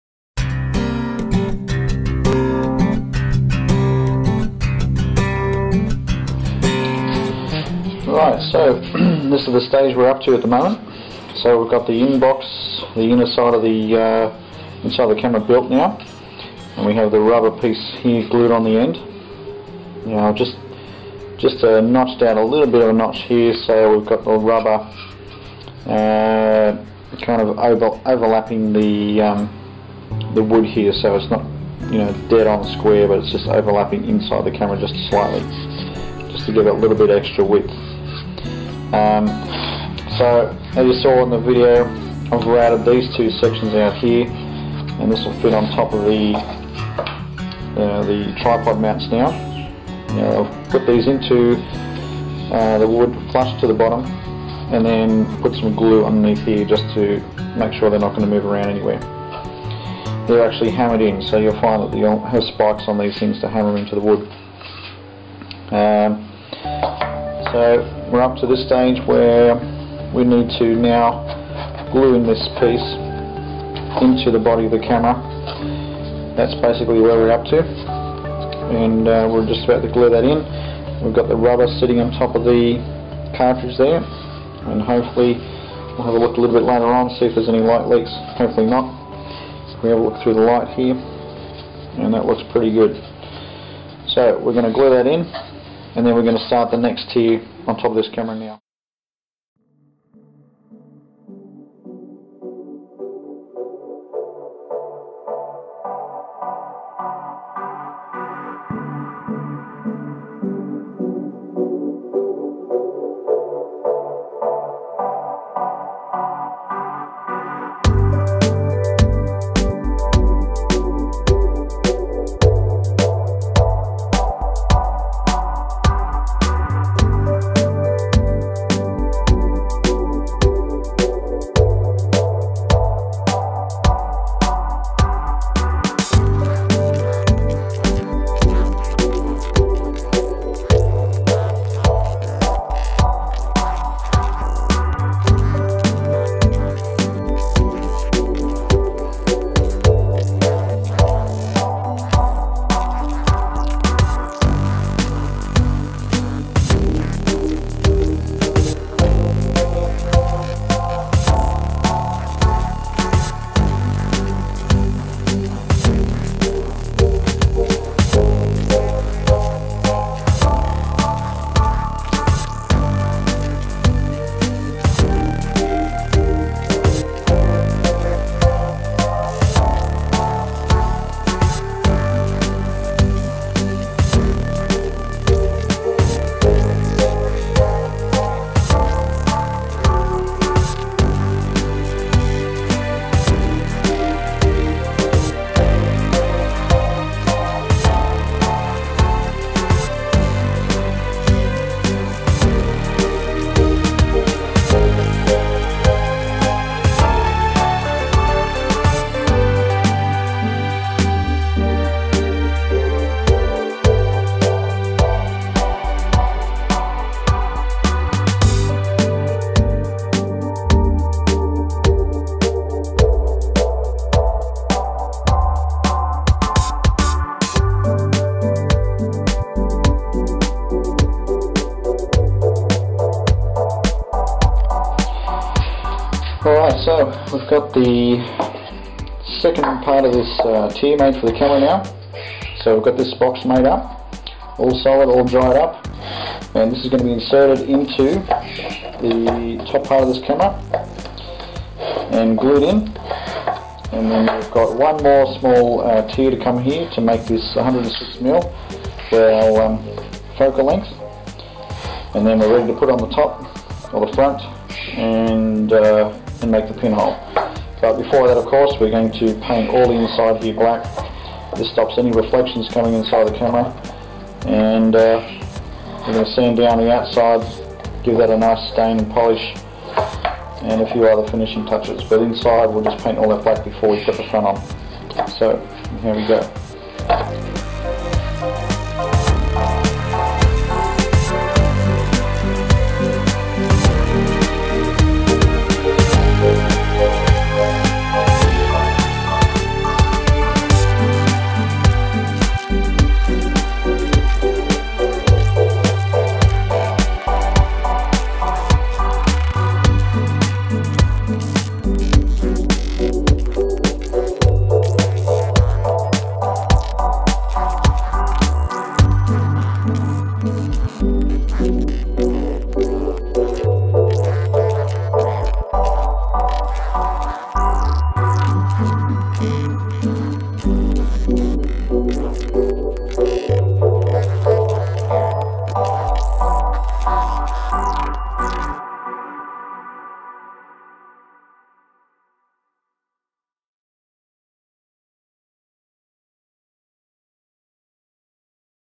Build a 4x5 Pinhole Camera - Part 6
Number 6 in a series of a DIY 4x5 pinhole camera built from materials found in the shed.